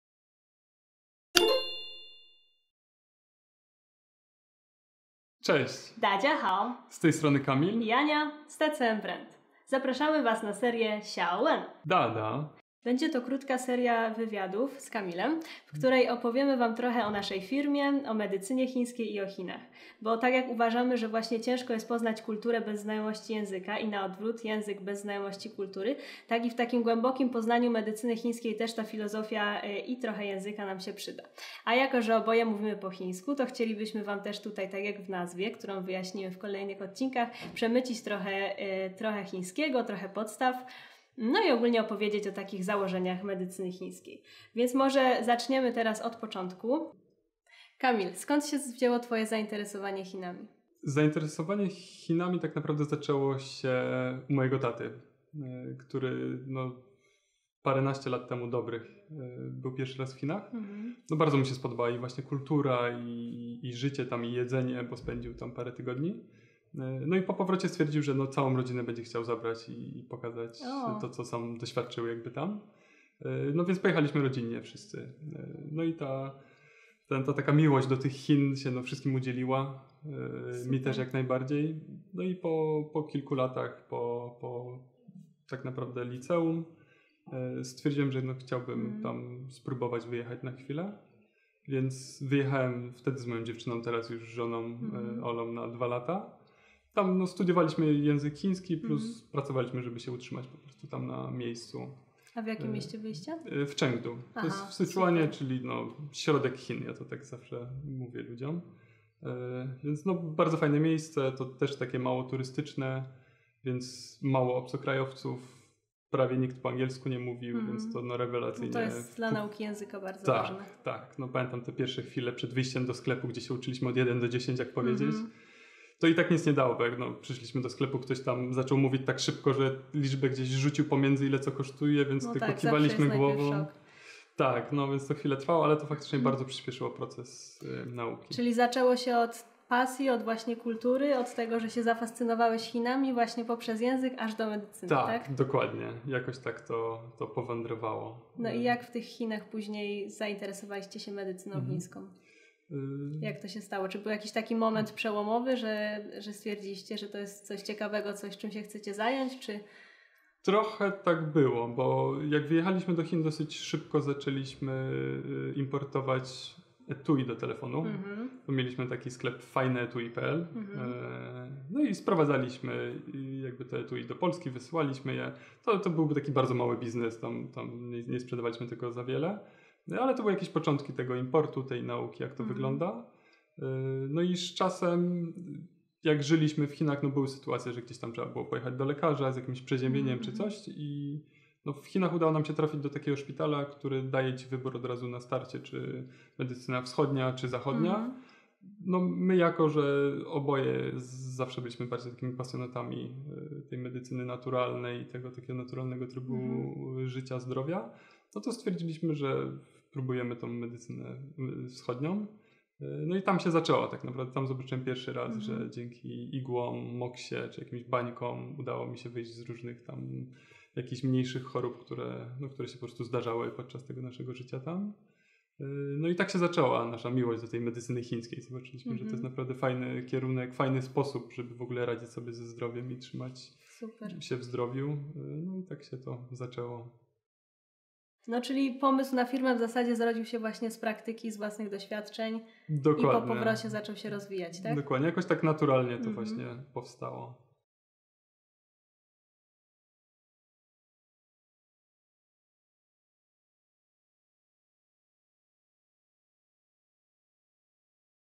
XIAO WEN DA DA 小问大答 | Odcinek #1 | Część #1
Zapraszamy Was na serię XIAO WEN DA DA 小问大答, czyli cykl rozmów o naszej firmie, TCM i medycynie naturalnej oraz o… języku chińskim!